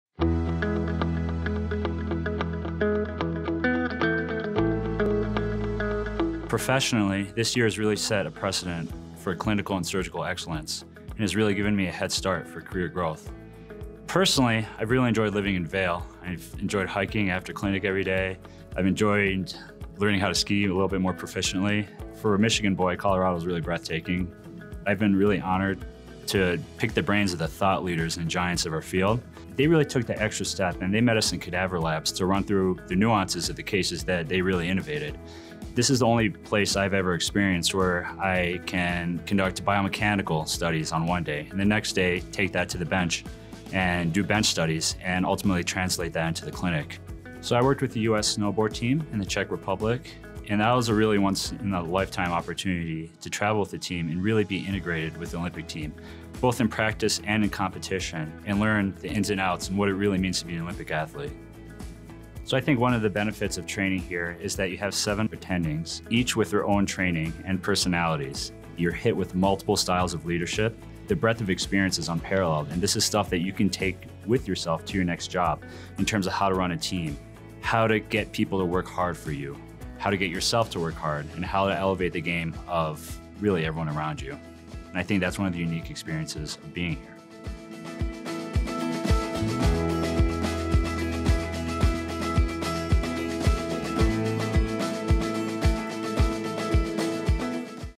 Hail & Farewell 2022 - Toufic Jildeh
The Steadman Clinic and Steadman Philippon Research Institute salutes Dr. Toufic Jildeh, who has completed his one-year Sports Medicine Fellowship in Vail!